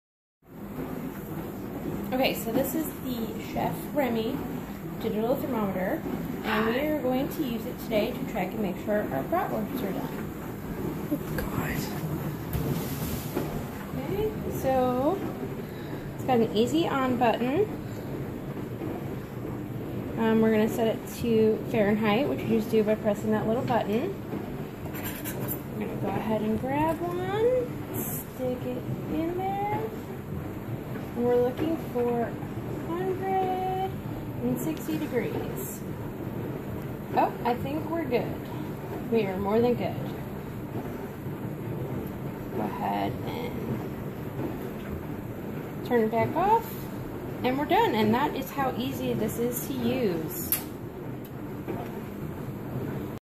Chef Remi Digital Thermometer Demonstration
A quick demo on my new Chef Remi Digital Thermometer!